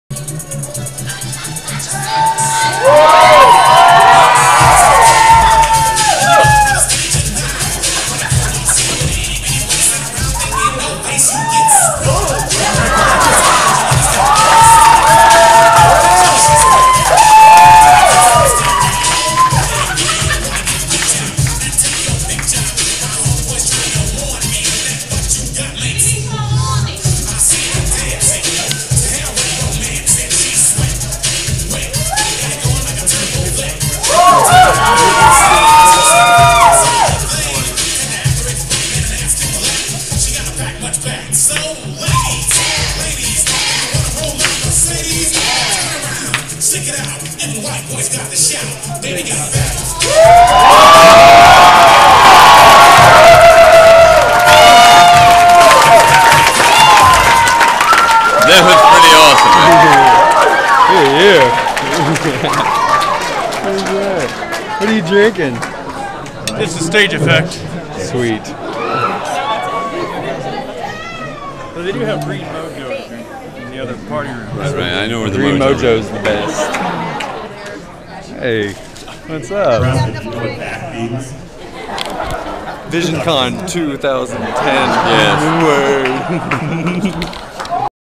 Winners of the Costume Contest - VisionCon 2010
Me and E-Man havin a Blast at VisionCon!  The coolest comicbook convention in Springfield, MO!